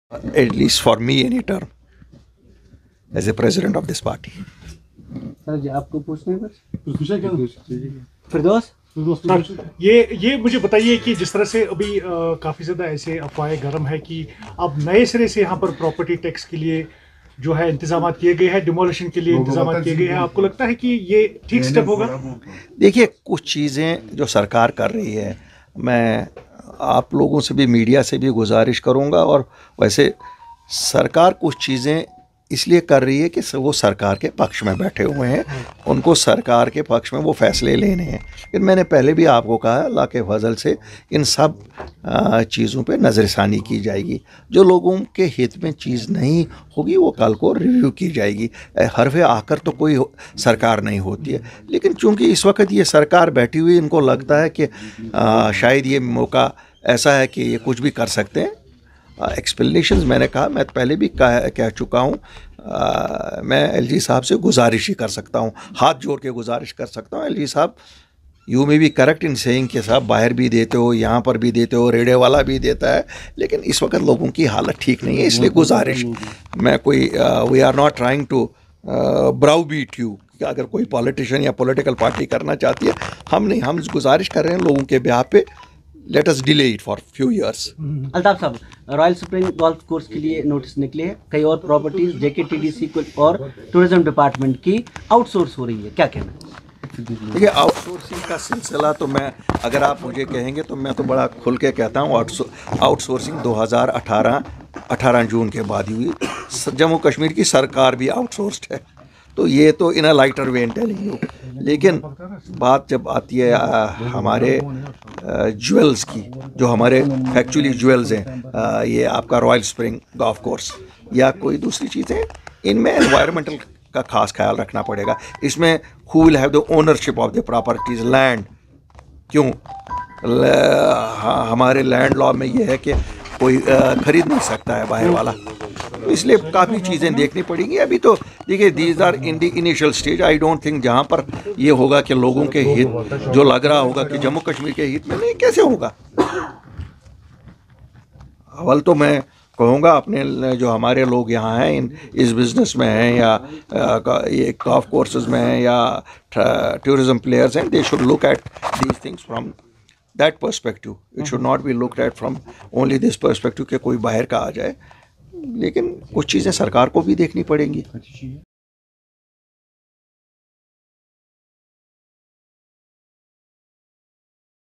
Altaf Bukhari Urges LG To Delay Property Tax
Apni Party chief Syed Mohammad Altaf Bukhari on Monday appealed Lieutenant Governor Manoj Sinha to delay the imposition of property tax by a few years